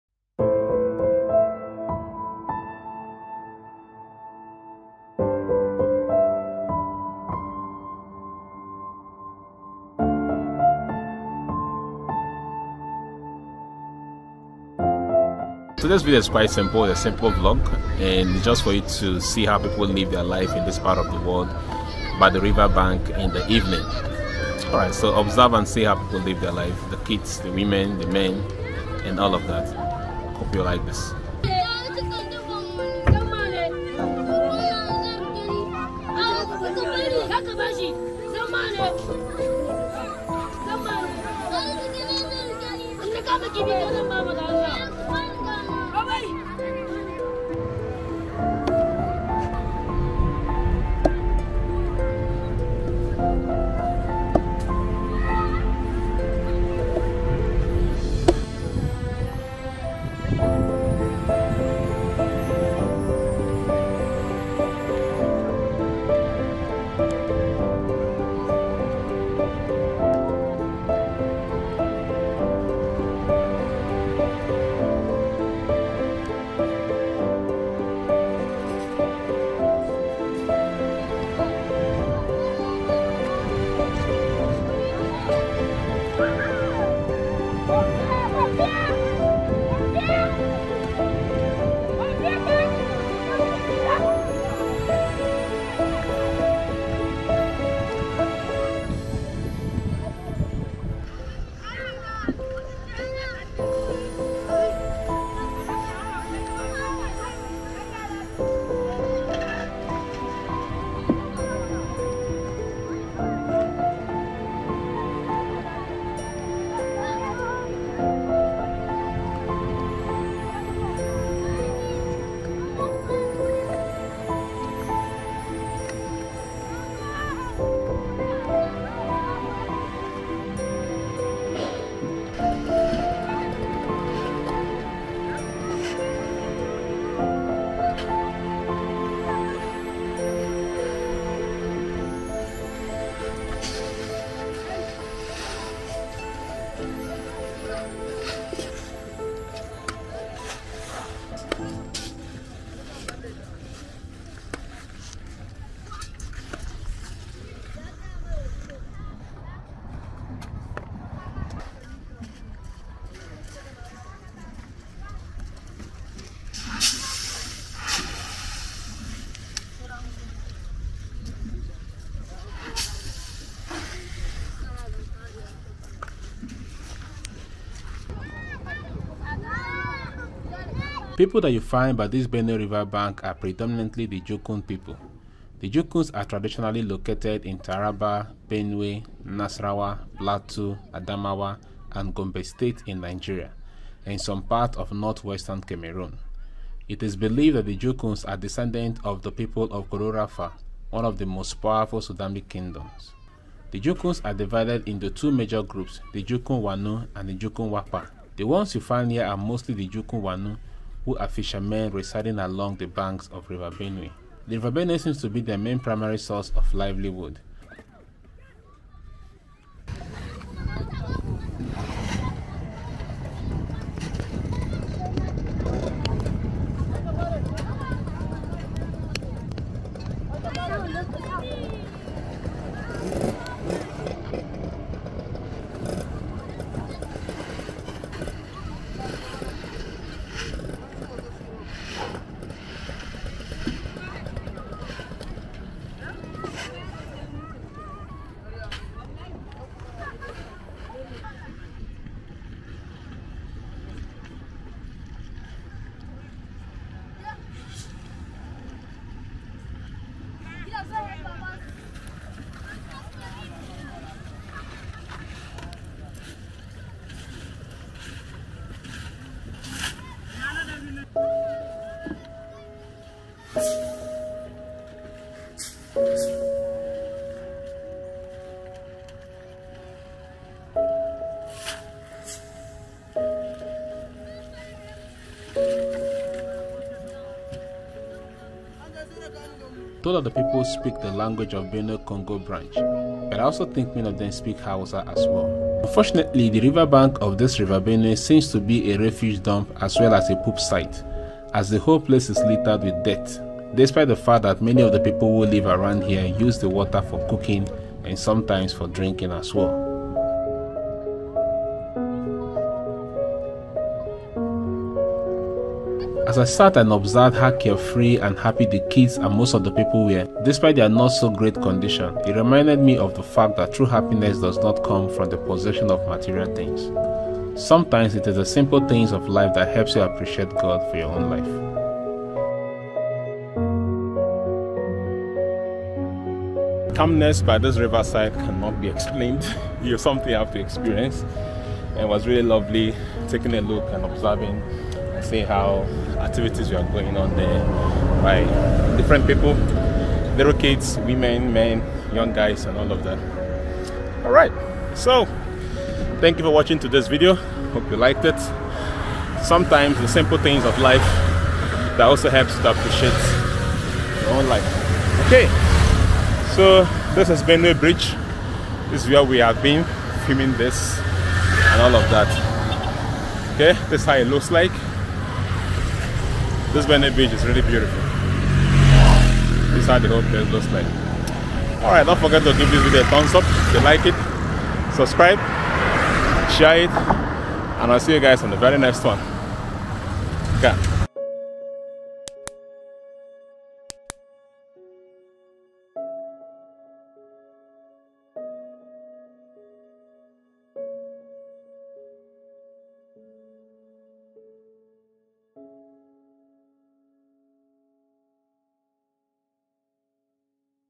The Nigeria RIVER BANK People - Part 1
The people you find by this benue river bank are predominantly Jukun people.
The Jukun are traditionally located in Taraba, Benue, Nasarawa, Plateau, Adamawa, and Gombe States in Nigeria and parts of northwestern Cameroon.
It is believed that the Jukuns are descendants of the people of Kororofa, one of the most powerful Sudanic kingdoms during the late European Middle Ages.
The Jukuns are divided into two major groups; the Jukun Wanu and Jukun Wapa.
This ones here are mostly the Jukun Wanu who are fishermen residing along the banks of the river Benue.
Am told that they speak a language of the Benue-Congo branch, but I also think many of them speak hausa as well.
The river Benue sems to be the primary source of livelihood.
The riverbank seems to also be a refuse dump and boof site, as the whole place is lilted with dirt, despite the fact that many of the people who live around here using the water for cooking and sometimes for drinking too.
However as I sat and observed how carefree and happy the kids and most of the people where, despite their not so great condition, it rememder me that true happiness those not comes from the possession of material things.
Sometimes it is the simple things of life that helps you to appreciate God for your own life.